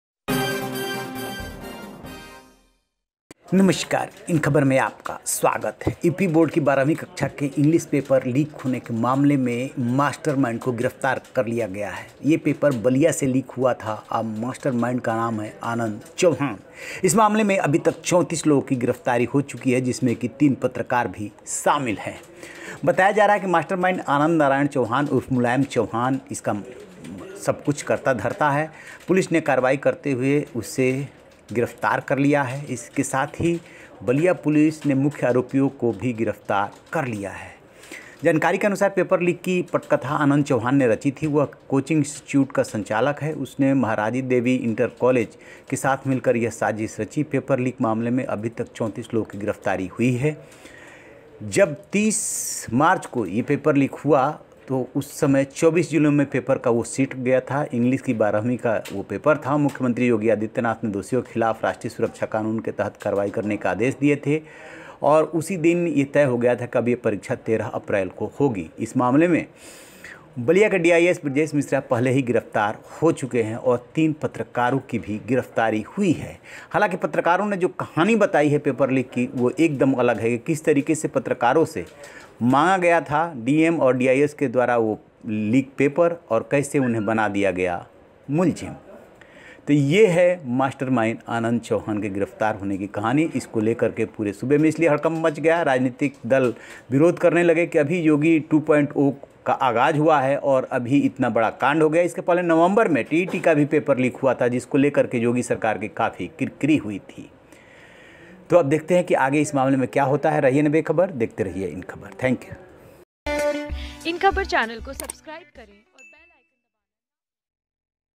UP Inter paper leak case : Mastermind Anand Chauhan alias Mulayam arrested, मास्टर माइंड गिरफ्तार
यूपी बोर्ड (UP Board) के 12वीं के इंग्लिश पेपर लीक (English Paper Leak) मामले में मास्टरमाइंड आनंद नारायण चौहान उर्फ मुलायम चौहान है. अब पुलिस ने कार्रवाई करते हुए आनंद नारायण चौहान को गिरफ्तार कर लिया है. इसके साथ ही बलिया पुलिस (Ballia Police) ने मुख्य आरोपी के साथियों को भी गिरफ्तार कर लिया है. अभी तक 3 पत्रकारों समेत इस मामले में 34 गिरफ्तारी हो चुकी है.